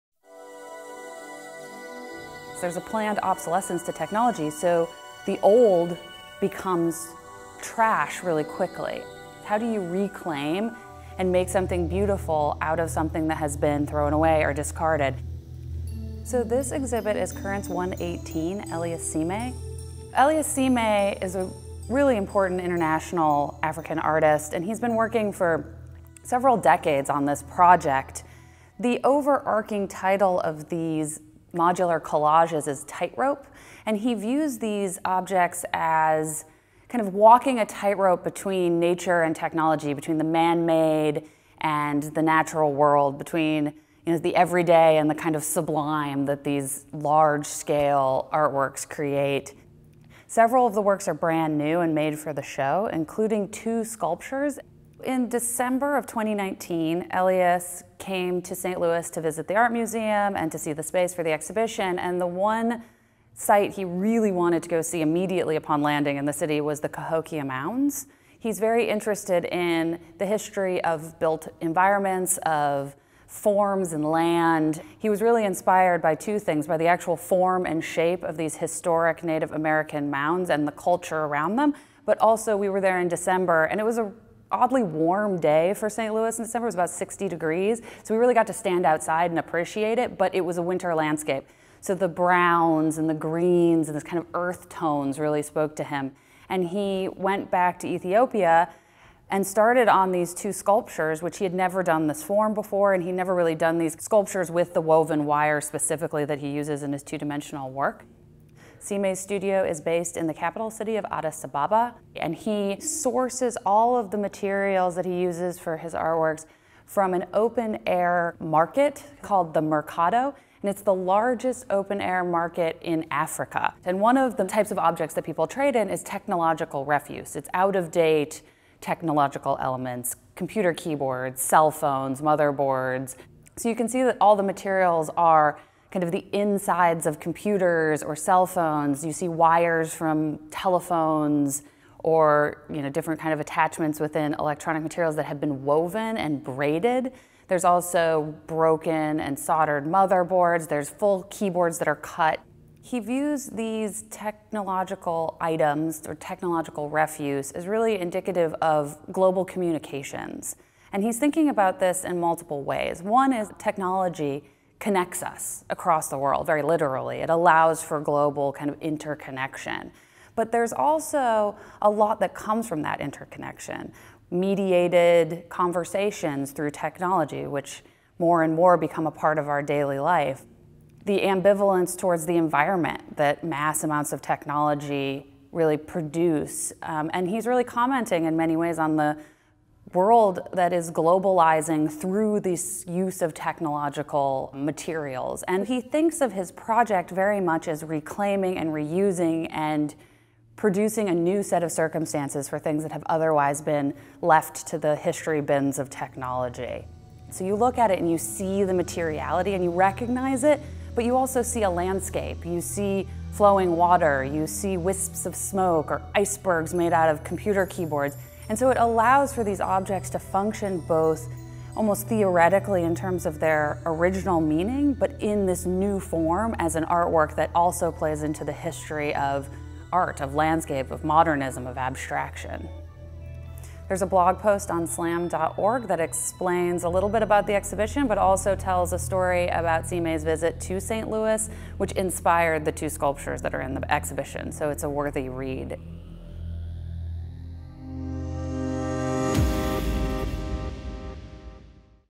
Elias Sime Crafts a New Narrative with Reclaimed Technology at SLAM
Currents 118 at the Saint Louis Art Museum brings the work of Ethiopian artist Elias Sime to Saint Louis.  Using salvaged materials, Sime’s creates large-scale modular works and sculptures that explore themes such as environmentalism, global communication, abstract art, and architecture.  Curator, Hannah Klemm, discusses the show and Sime's visit to the area which inspired two new sculptures.  The exhibition will be available through January 31, 2021.